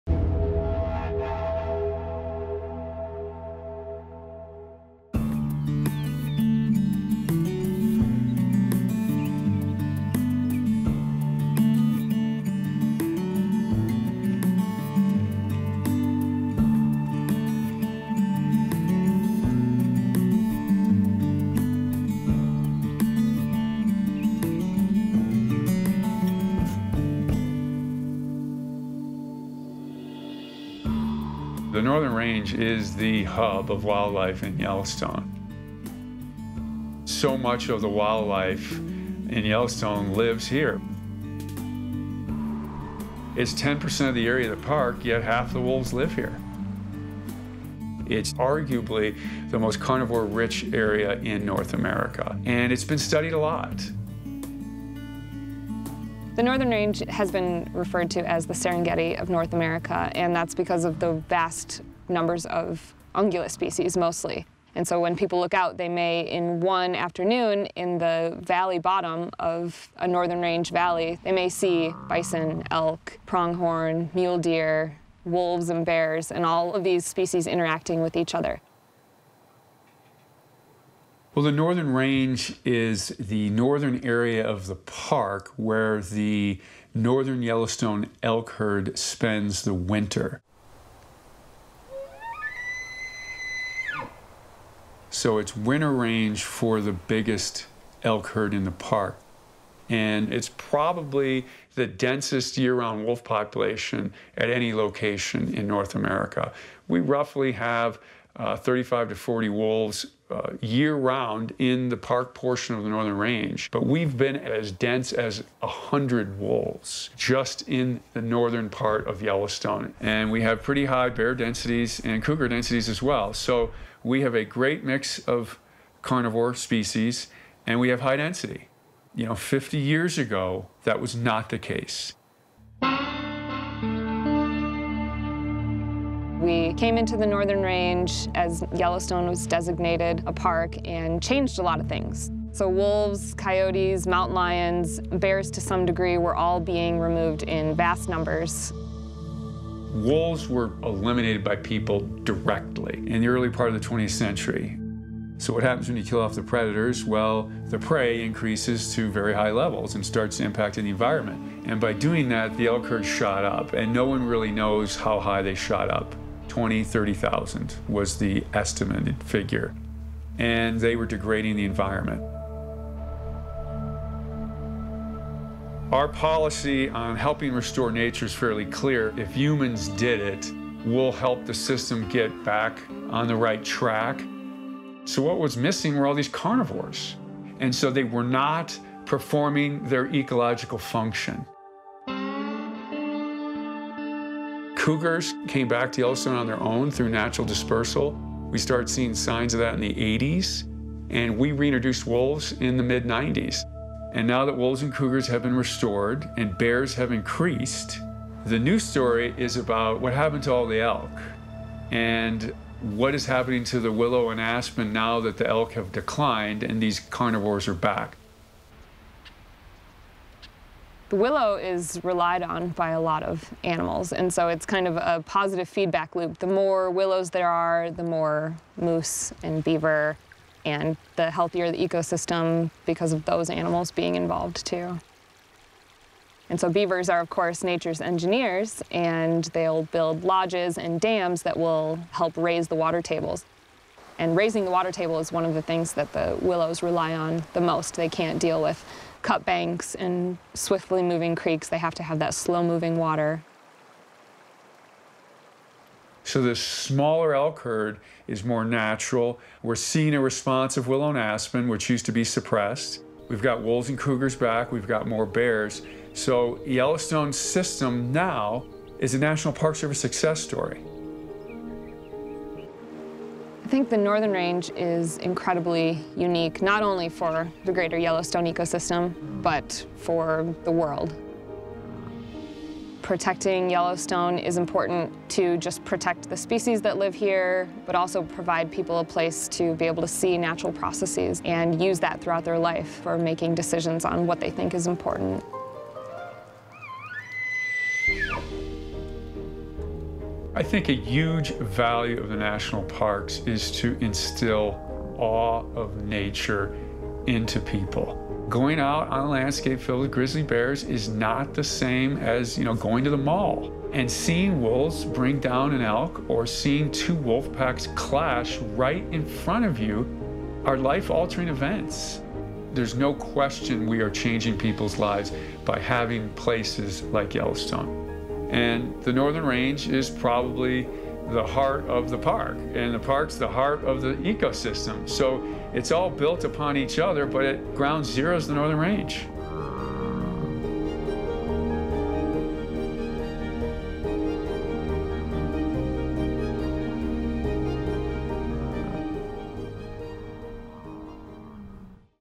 InDepth - The Northern Range
The Northern Range is the hub of wildlife in Yellowstone National Park. Occupying just 10 percent of the park, it is winter range for the biggest elk herd in Yellowstone and is arguably the most carnivore-rich area in North America. Early management of predators caused dynamic changes to the ecosystem. The reappearance of carnivores on the landscape has had significant and sometimes unexpected impacts on the resident grazers and their habitat.